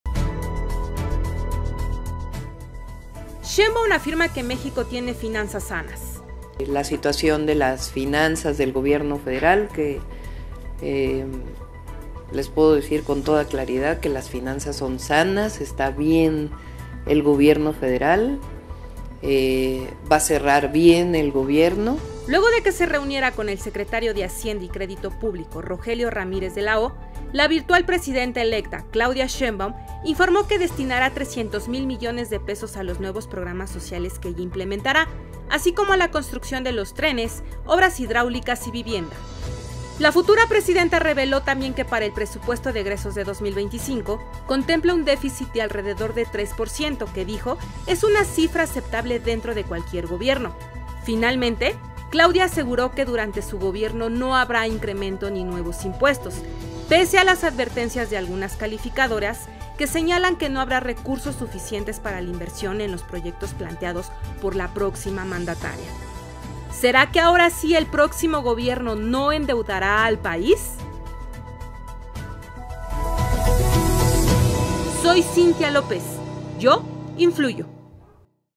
Sheinbaum afirma que México tiene finanzas sanas
La futura presidenta aseguró que seguirá con la política de austeridad implementada por el actual mandatario porque eso genera recursos para invertirlos en el pueblo de México.